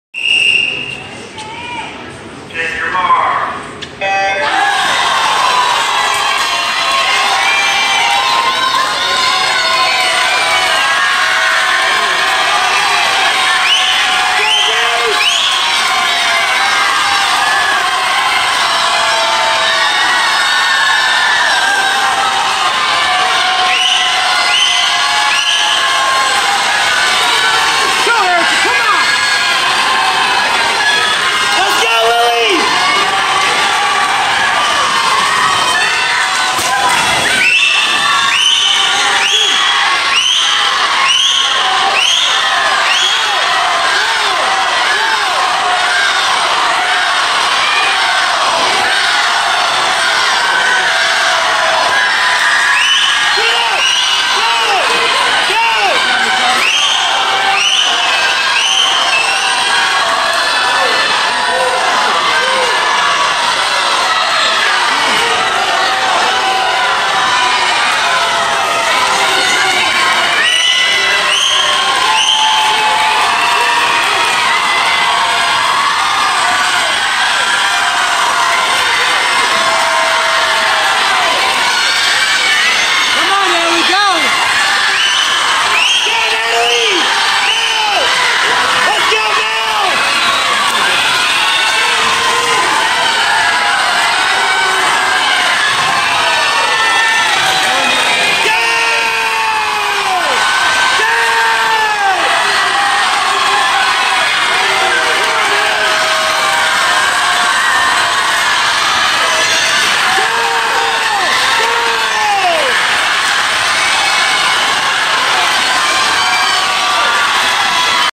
NHswimming NCL girls 200MR
Gilmour wins girls 200 medley relay Jan. 26 during the NCL meet at Gilmour.